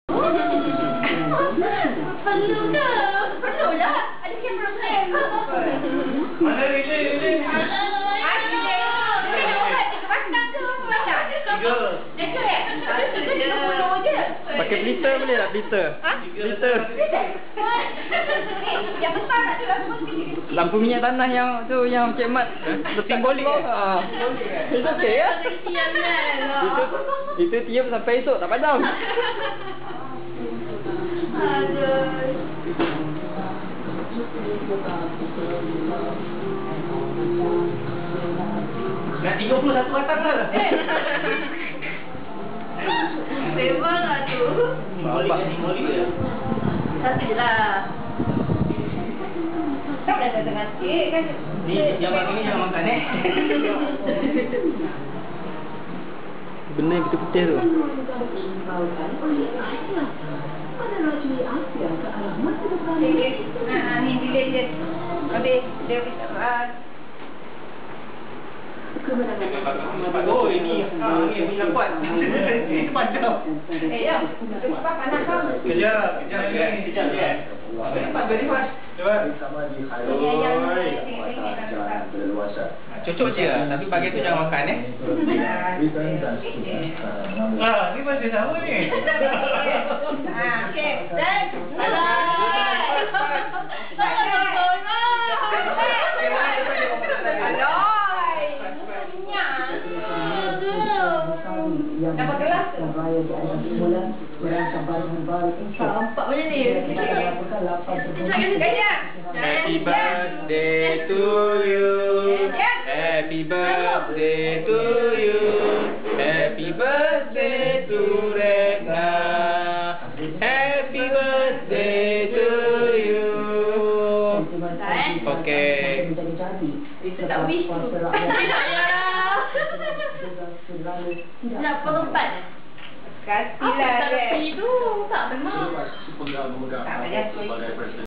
Besday Retna
ader makan-makan sempena besday retna kat rumah CM & CT